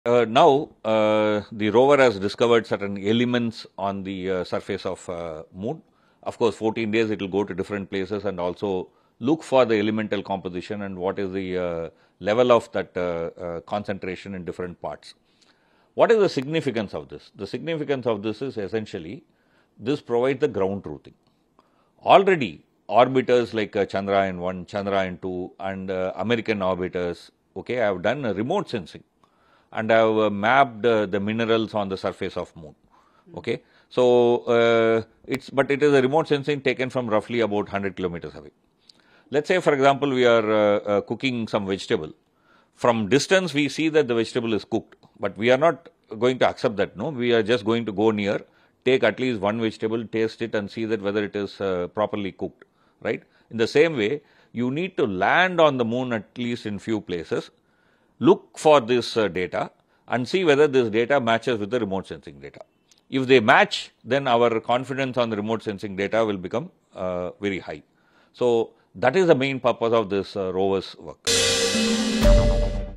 Chandrayaan-3’s In-Situ Tests On Moon Significant Than Other Remote Sensing Missions- Top Scientist
Chandrayaan-3’s Elemental Tests On Moon Surface Is Significant Than Other Remote Sensing Missions